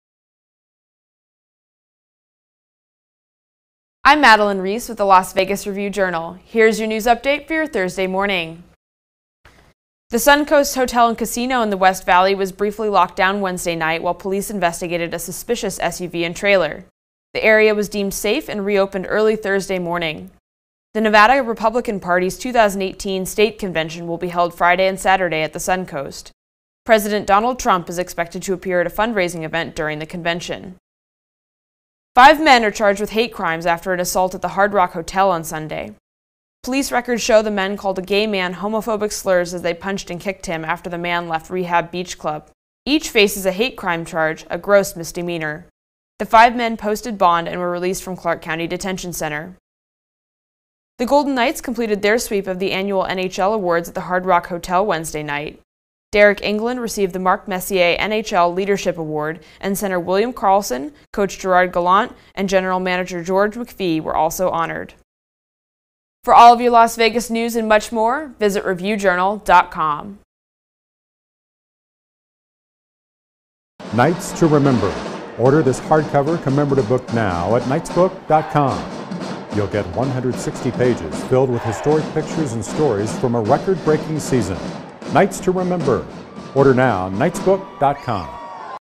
Las Vegas Morning Update - Thursday, June 21
Here are your Thursday morning headlines.
1. Vehicle cleared after lockdown at Suncoast in west Las Vegas
2. 5 charged with hate crime in assault at Las Vegas hotel.
3. Golden Knights complete sweep of NHL awards.